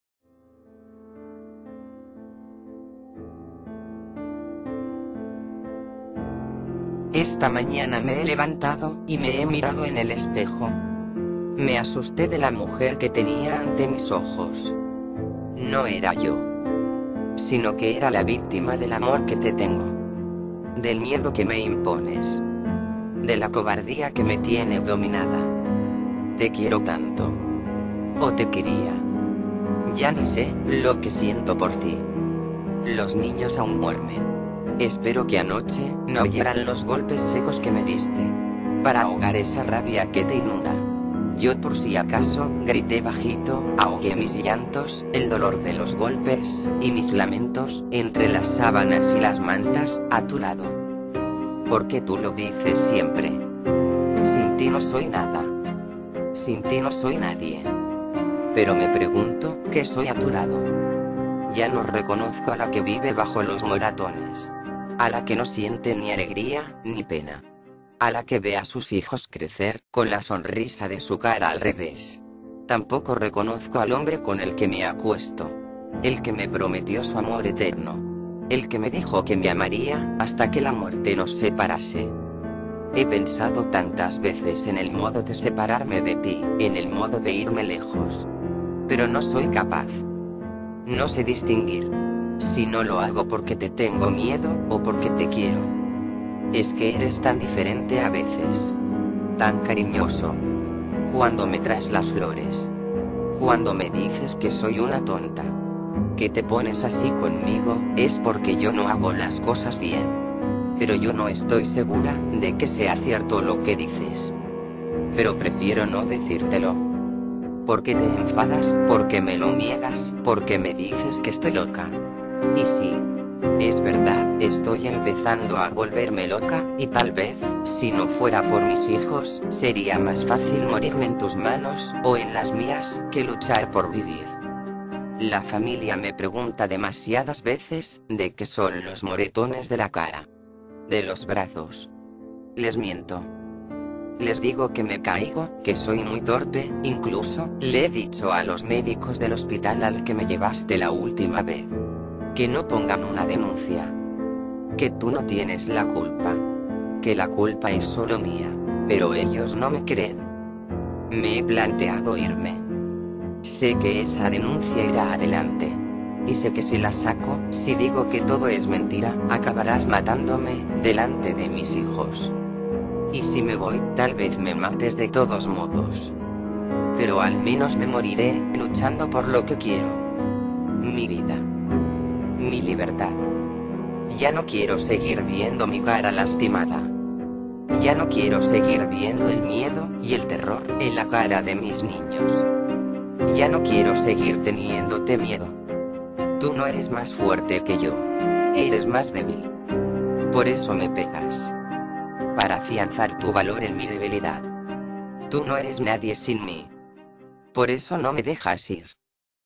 Mujer maltratada
La violencia contra la mujer es todo tipo de violencia ejercida contra la mujer por su condición de mujer.
Cuando la sangre de una mujer es maltratada, la herida es de todos.